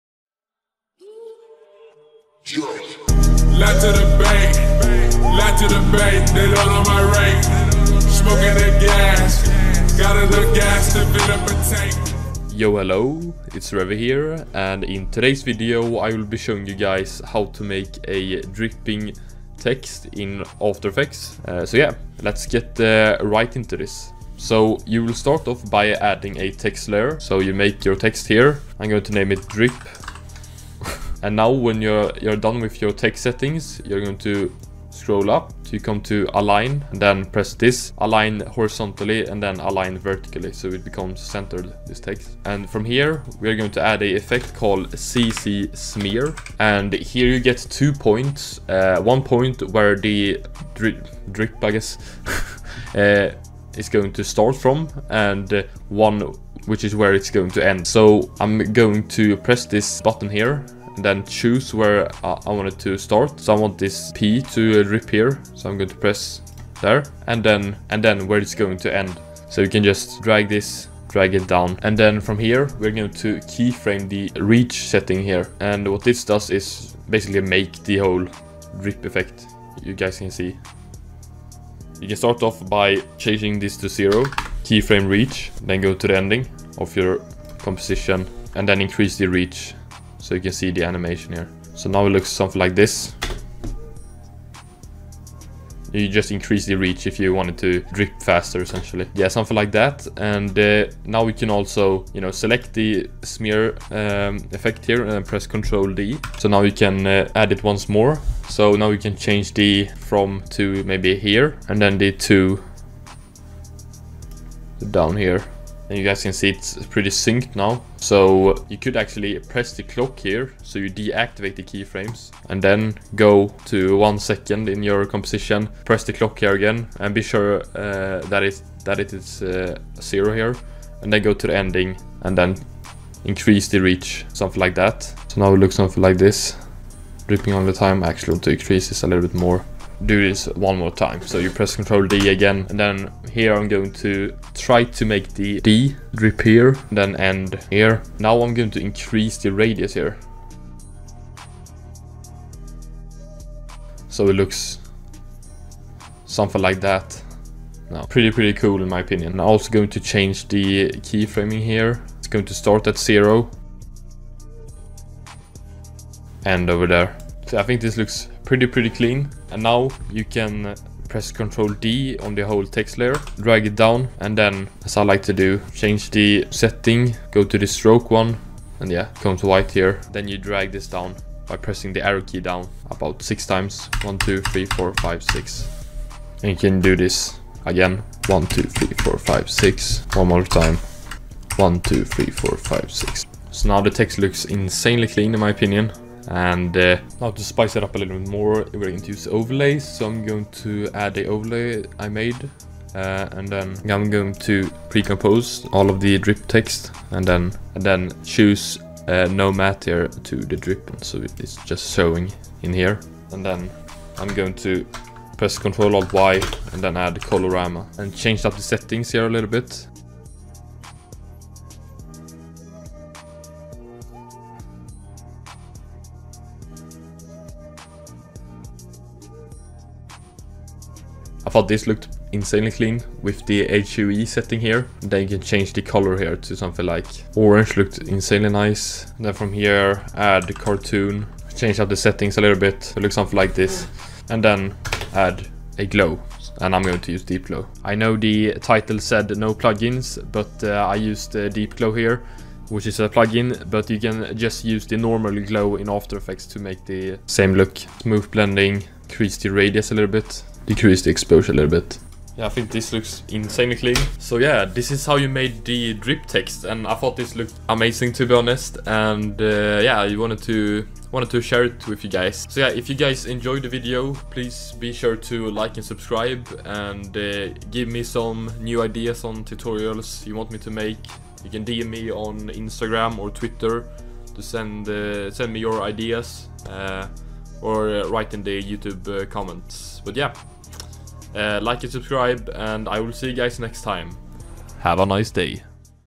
DRIPPING TEXT - After Effects Tutorial (NO PLUGINS)
This is one of my first tutorials guys so feel free to give me some feedback. In todays video I show you guys how to make a DRIPPING TEXT in after effects. Hope you guys enjoyed the video! Love Y'all 💙